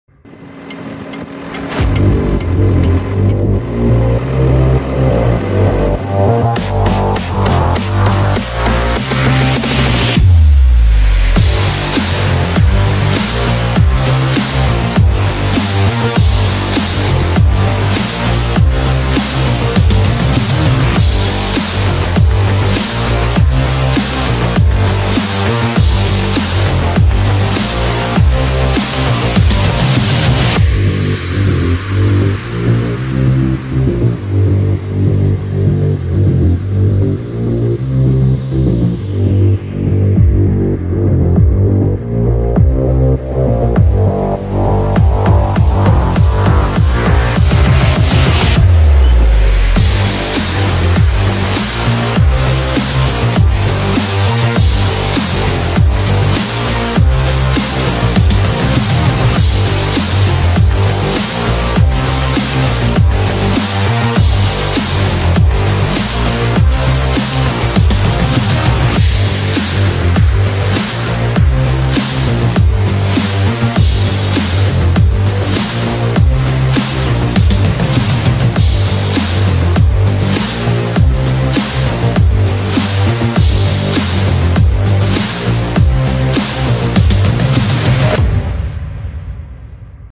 Talerzowanie 2021 z Nowym Nabytkiem ☆ New Holland T7.210 & Lemtech Colter
✩ Włącz dzwoneczek 🔔🔔
✩ Jeśli się spodobało daj łapkę w górę
✩ Chcesz więcej zostaw suba

Maszyny :
New Holland T7.210 & Lemtech Colter

Nagrywam:
✩ Lumix G80
✩ Lumix 12-60